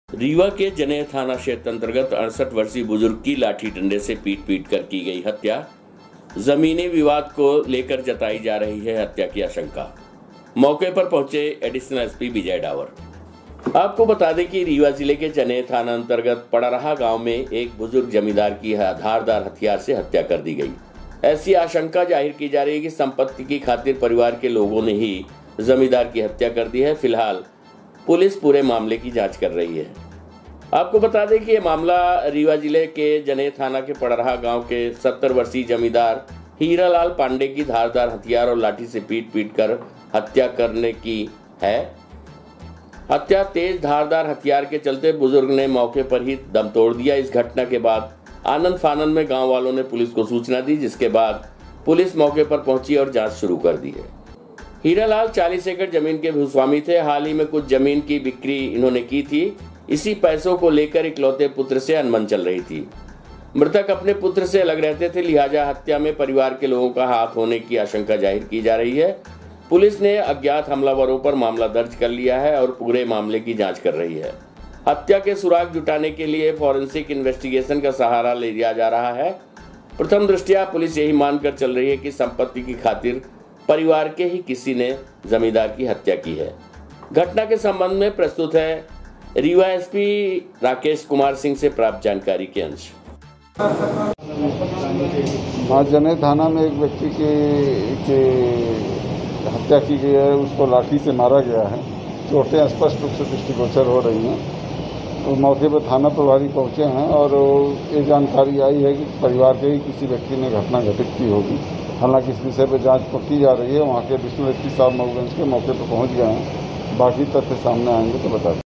बुजुर्ग जमीदार की धारदार हथियार से हत्या,संपत्ति के खातिर पुत्र पर संदेह,,पुलिस कर रही जांच,,,देखें
*बुजुर्ग जमींदार की धारदार हथियार से हत्या,संपत्ति के खातिर पुत्र पर संदेह,,पुलिस कर रही जांच,,,,देखें सिटी न्यूज़ वैन की यह खबर।*